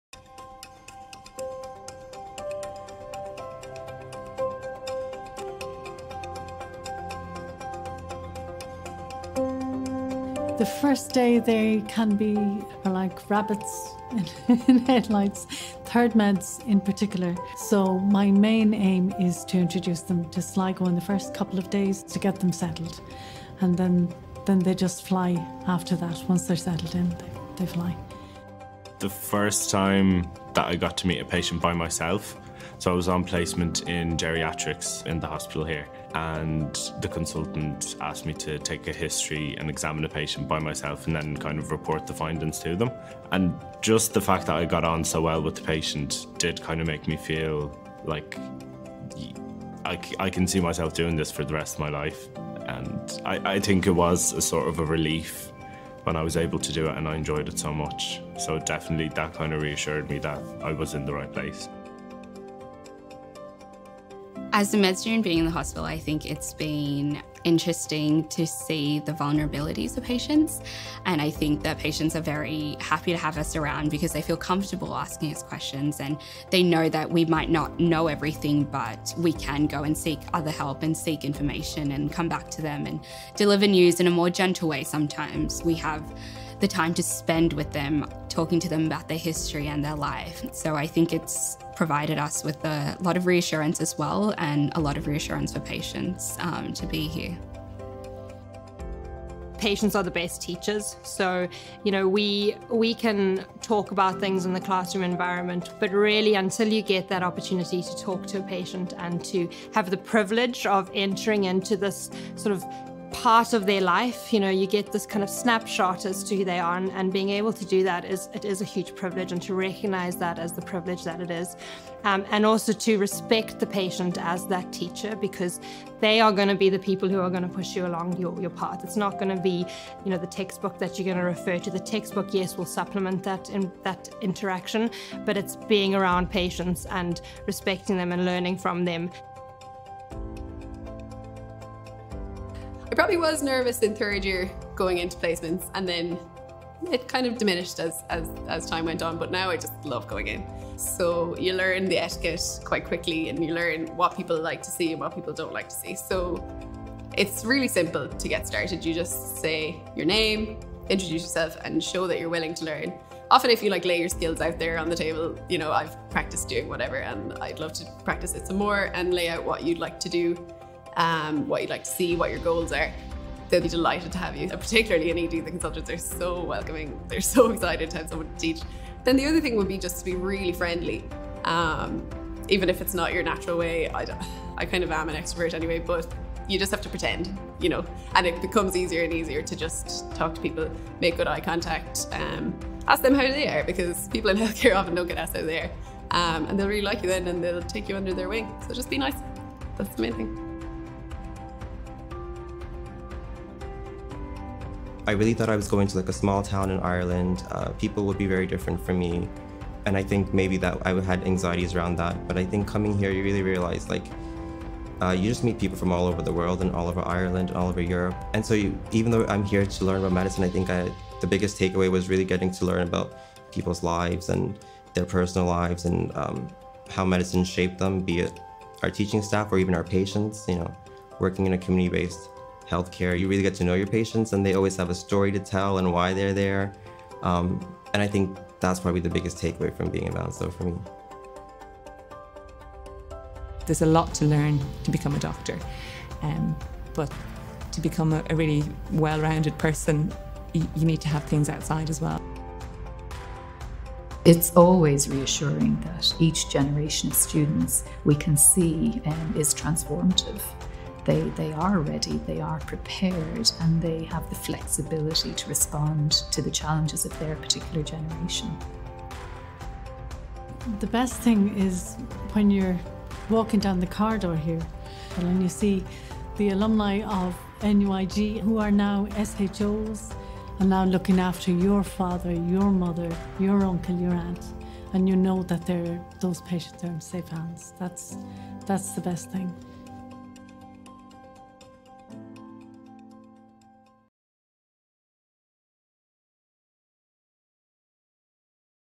Medical academies video
Explore University of Galway's medical academies in Letterkenny, Sligo, Castlebar, and Ballinasloe.

In the clinical years at University of Galway, students relocate from Galway to spend a full calendar year at one of our academies. Each academy has a purpose-built facility of the highest international standards of medical education, comprising lecture halls, tutorial rooms, clinical skills labs and student chat/social spaces.

These academy buildings are located on Saolta University Hospital sites, ensuring ease of access from the hospital to teaching and learning facilities. Students gain a unique experience of service delivery in a secondary health care centre.

Each academy has dedicated academic and administrative staff, under the direction of an academy Dean, with excellent student-staff ratios. Small student numbers enhance teaching and mentoring from clinical staff and facilitate patient contact.